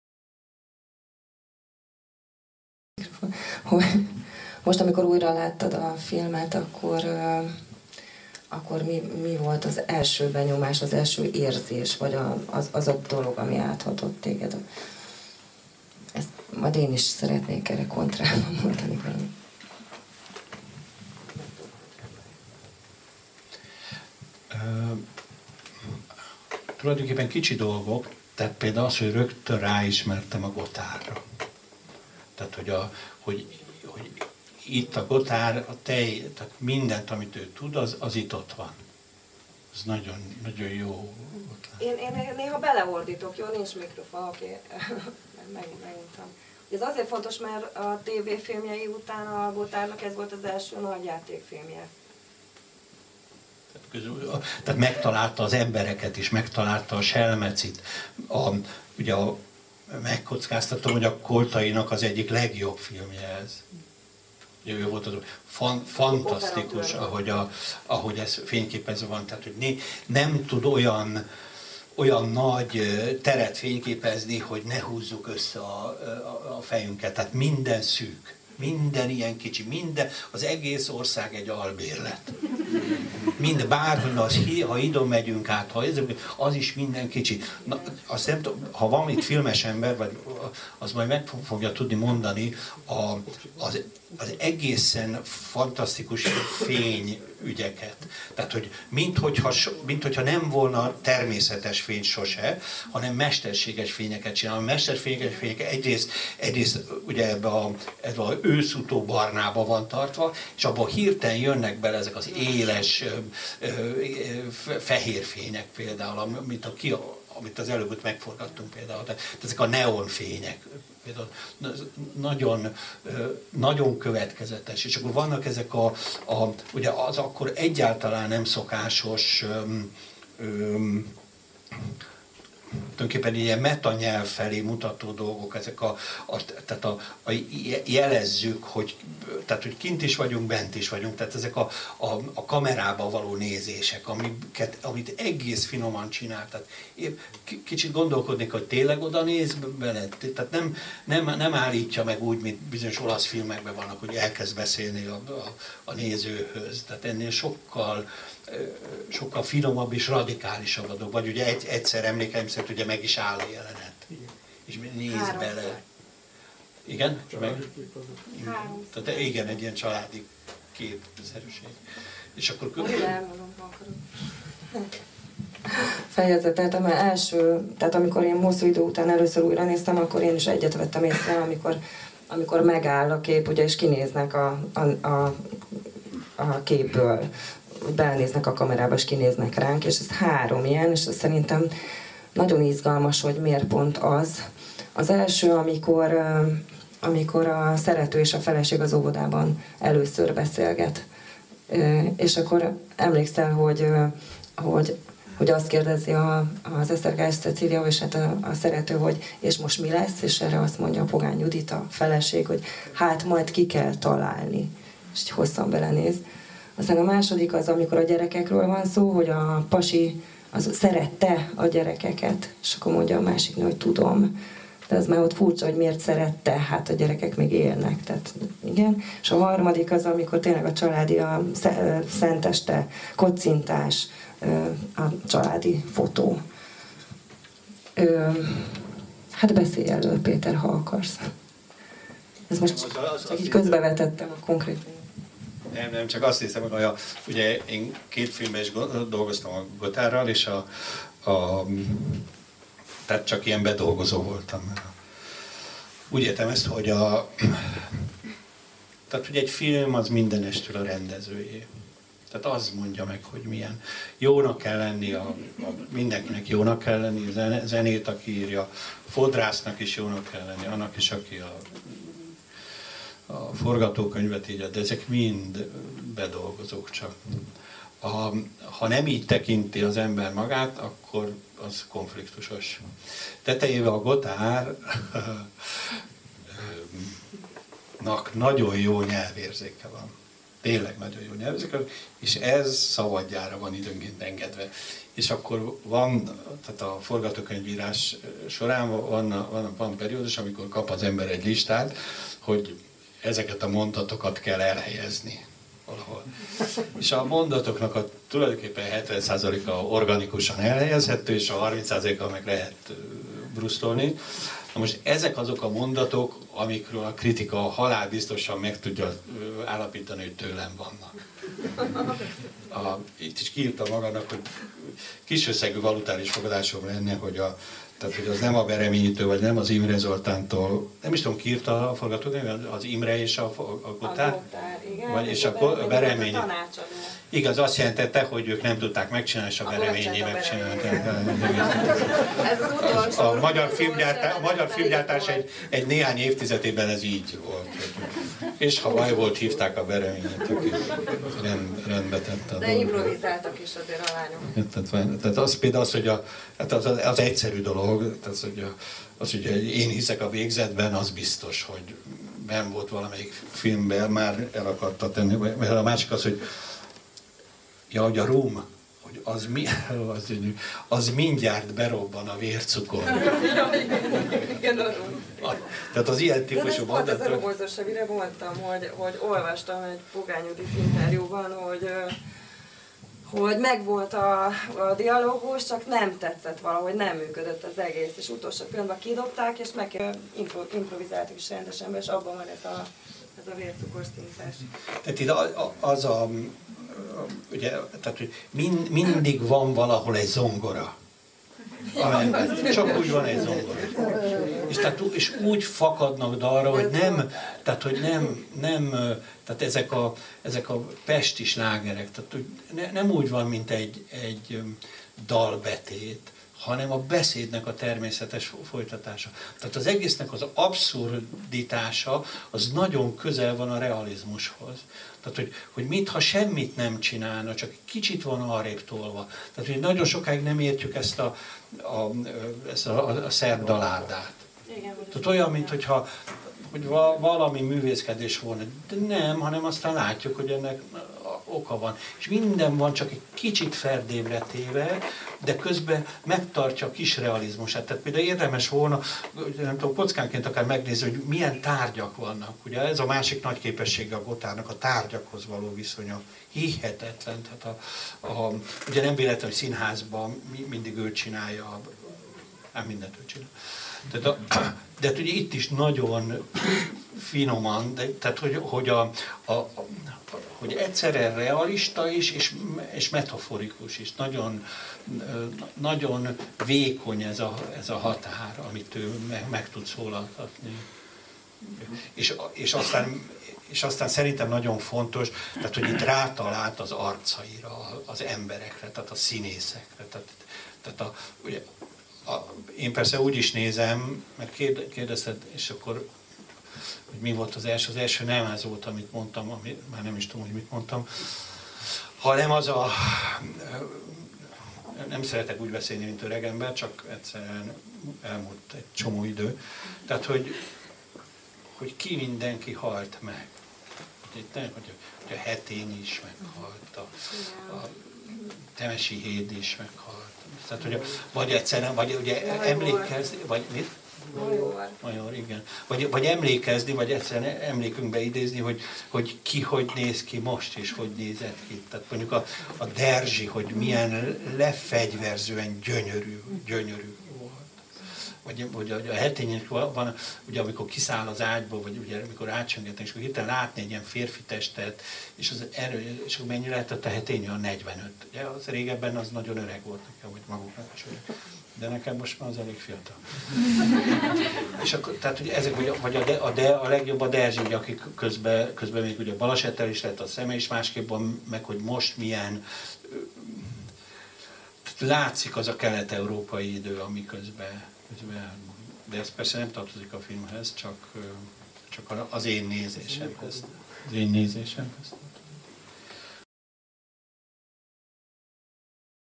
Ajándék volt ez a nap Esterházy Péterrel
A Litera és a Nyitott Műhely közös filmklubjának, az Írómozinak első vendége február 23-án Esterházy Péter volt, akivel egyik kedvenc filmjét, Gothár Péter "Ajándék ez a nap" című alkotását nézhettük meg. A film utáni beszélgetésről készült videót ajánljuk most. (2011)